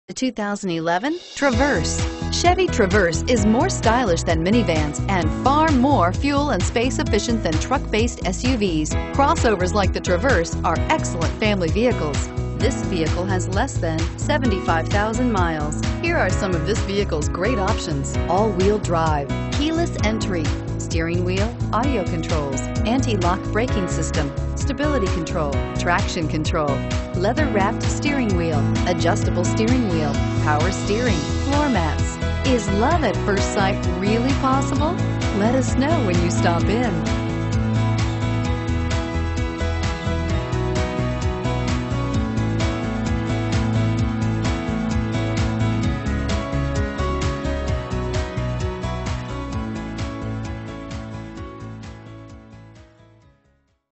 2011 Chevrolet Traverse 3J160017A
2011 CHEVROLET TRAVERSE Willmar, MN
Stock# 3J160017A

*This 2011 Chevrolet Traverse LT w/1LT will sell fast -3rd Row Seating Multi-Point Inspected -Aux. Audio Input ABS Brakes -Power Seat -Automatic Headlights -AM/FM Radio Based on the excellent condition of this vehicle, along with the options and color, this Chevrolet Traverse is sure to sell fast. -Multi-Zone Air Conditioning *Multi-Point Inspection and many other amenities that are sure to please.